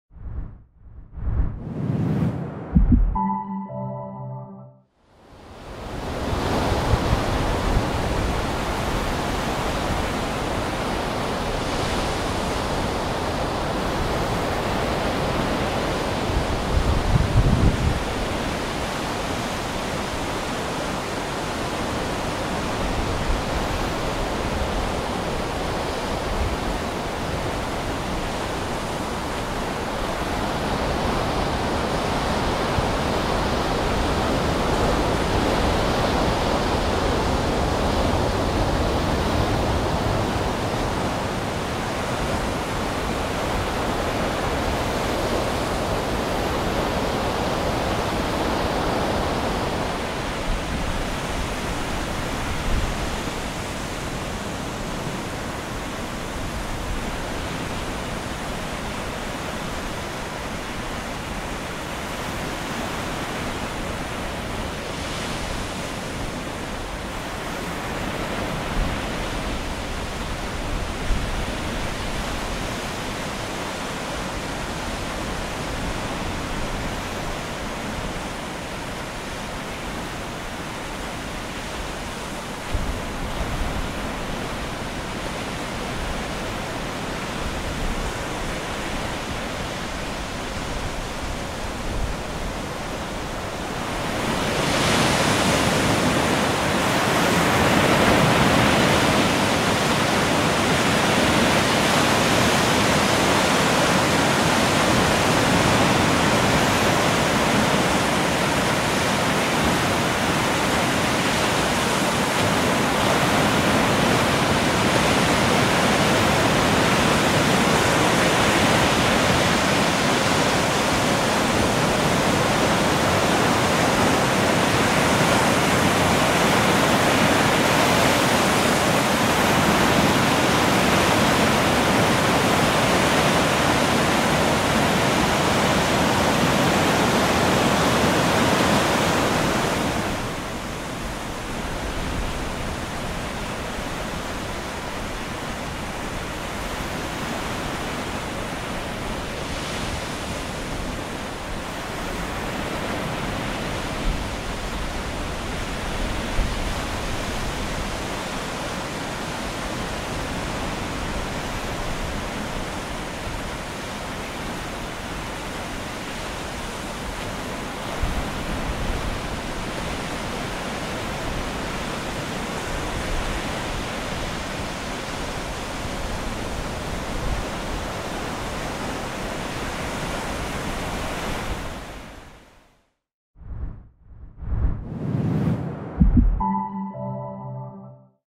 Northern California Coast in VR
Come with us as we journey along the rugged Northern California coastline and explore this unique and pre-historic land. From Wedding Rock—the major landmark on the large rocky headland of Patrick’s Point at Patrick’s Point State Park, to the 115-foot Point Arena Lighthouse which was built after the first one was destroyed in the 1906 earthquake to the scenic Trinidad State Beach featuring the offshore rocks of the California Coastal National Monument and colorful tide pools providing specimens to Humboldt State University’s Marine Laboratory.